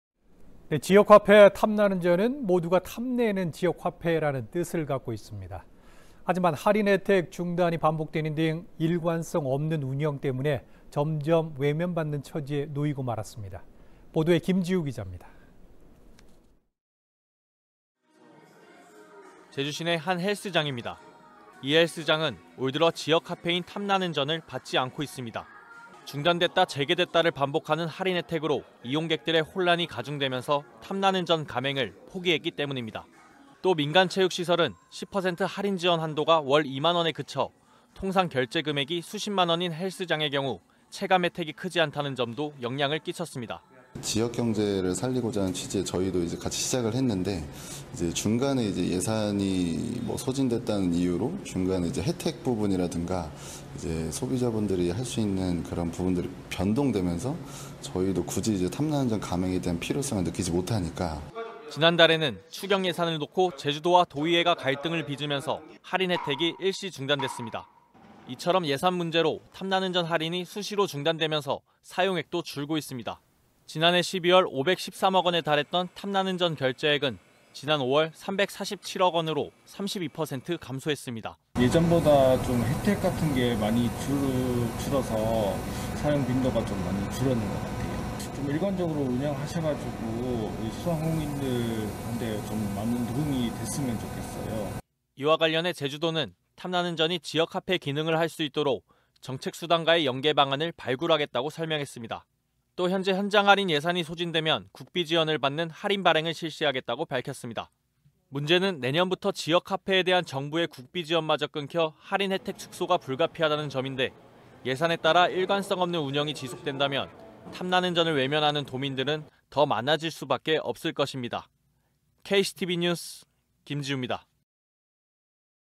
"툭하면 할인 중단"…탐나지 않는 '탐나는전'
지역화폐 탐나는전은 모두가 탐내는 지역화폐라는 뜻을 갖고 있습니다.

하지만 할인 혜택 중단이 반복되는 등 일관성 없는 운영으로 점점 외면받는 처지에 놓이고 있습니다.